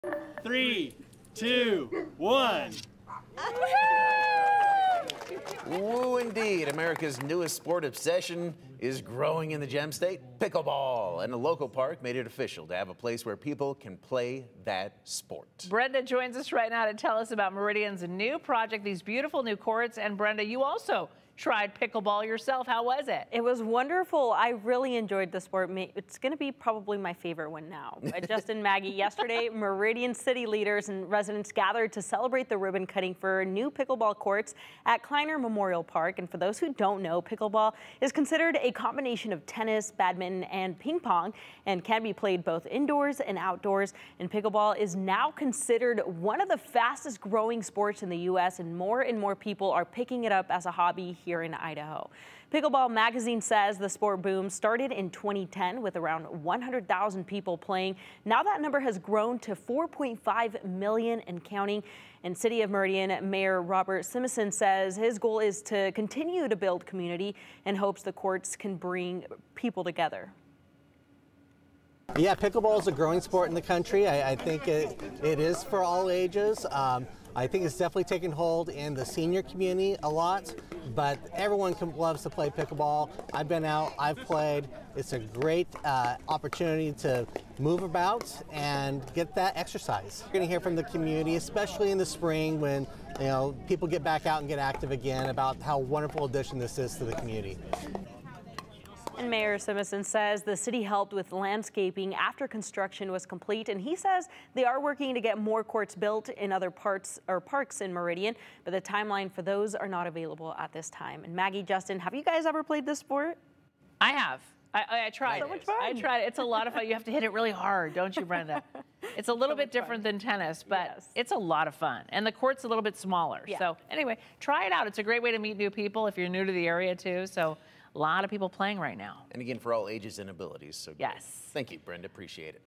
New pickleball courts open at Meridian park
The fast-growing sport, suitable for all ages, now has a home at Kleiner Memorial Park.